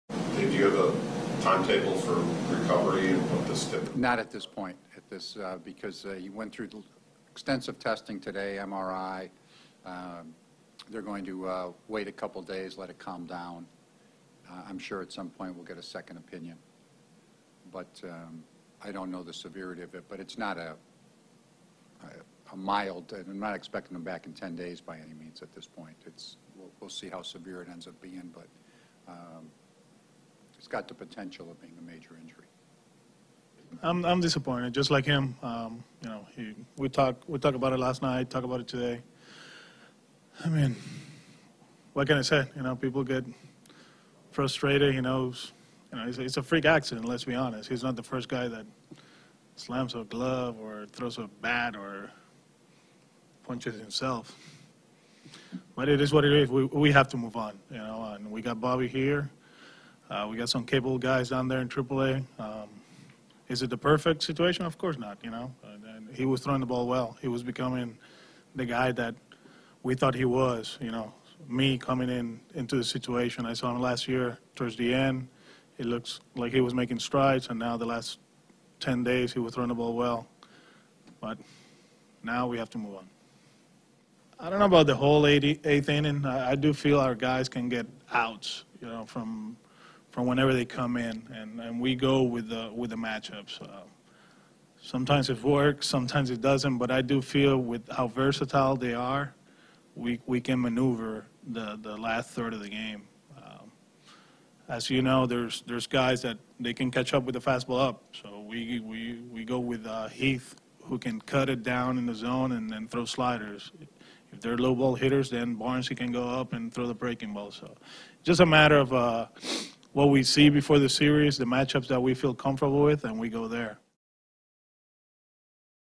Alex Cora, Dave Dombrowski Comment On Carson Smith's Freak Injury
Boston Red Sox manager Alex Cora and President of Baseball Operations Dave Dombrowski both commented on Carson Smith's freak injury at Fenway Park on Tuesday.